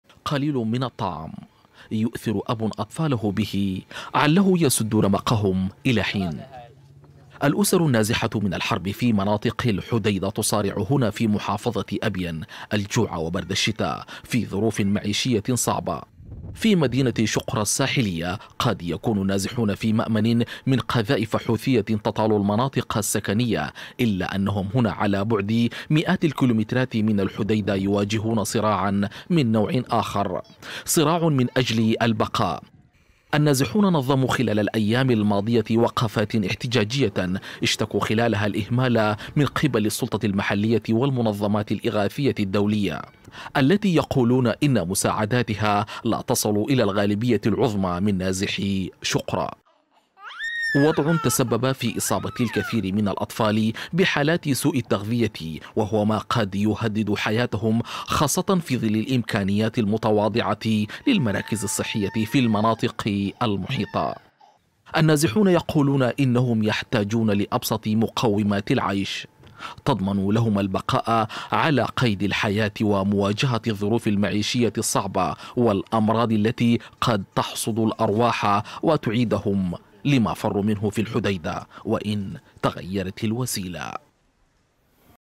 نازحو الحديدة يواجهون أوضاعا معيشية صعبة في محافظة أبين
تعاني مئات الاسر النازحة من الحديدة الى مخيمات في محافظة ابين جنوب اليمن ، جراء الاوضاع المعيشية الصعبة خاصة مع عدم وصول مساعدات المنظمات الاغاثية والانسانية التي يقول النازحون انها لا تصل الى مناطقهم